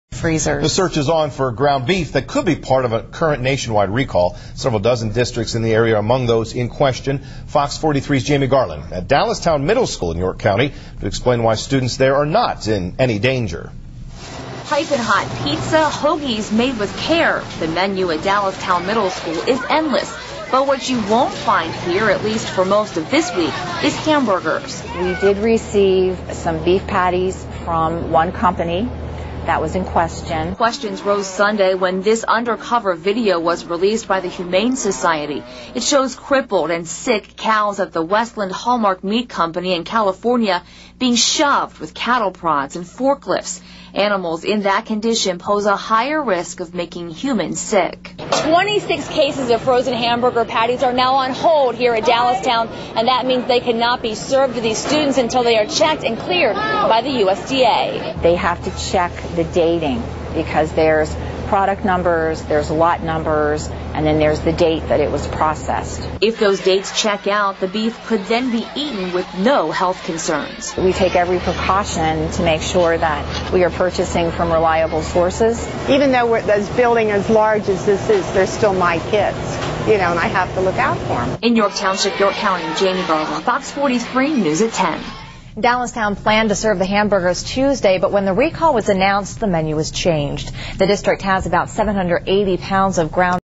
Beef from Sick Cows Sold to Schools
Beef from sick cows was sold to schools and is now being recalled.  Video shows mistreatment of injured animals.